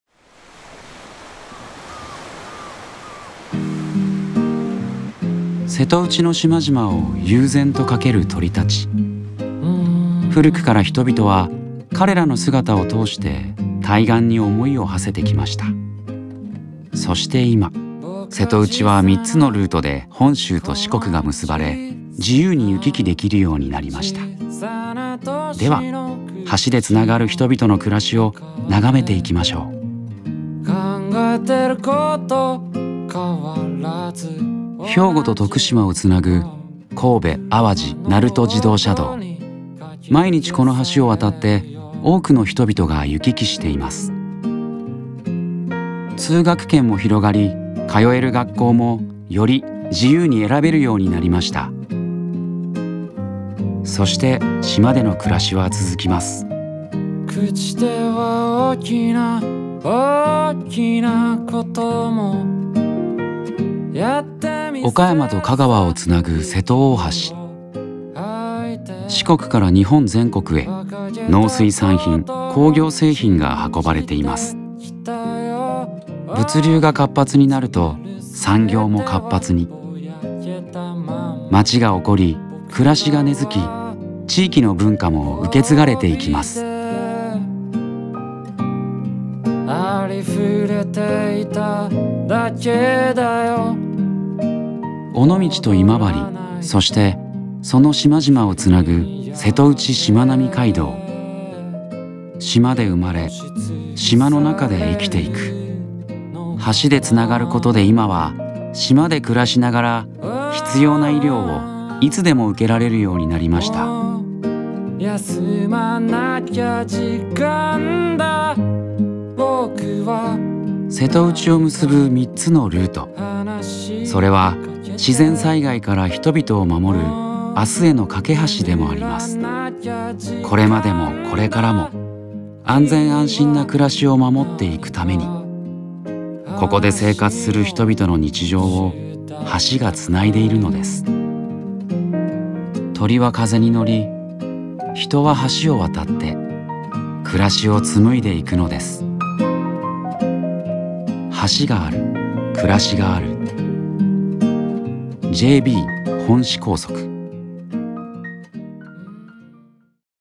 本四高速道路の整備効果PR映像「せとうち鳥瞰」
地域の生活環境や持続的な地域社会の実現を支える本四架橋の役割について、「教育」「産業」「医療」「安全・安心」の４つの視点から紹介します。